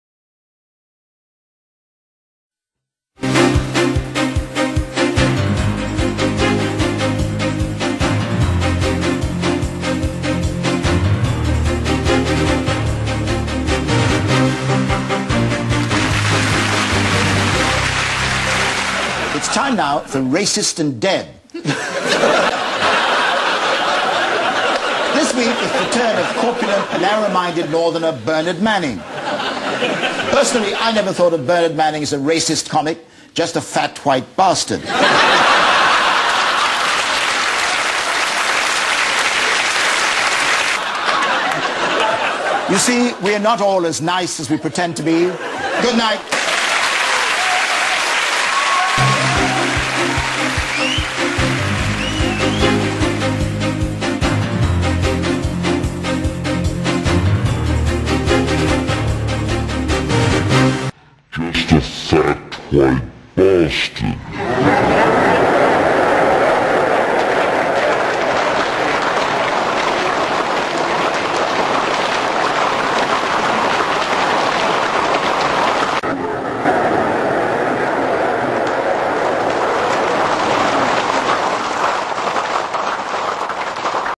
Sir Trevor McDonald Racism Incident/Joke
TV watchdog Ofcom has cleared ITV of racism after Sir Trevor McDonald branded the late controversial comic Bernard Manning a " fat, white bastard " on his satirical game show.
More than 100 people complained that the comment, made the week that Manning died, was racist.
Ofcom said Sir Trevor " clearly intended to parody " Manning's own brand of comedy, which played on racial stereotypes and was often denounced as racist. The watchdog said that McDonald's comments were "justified by the context".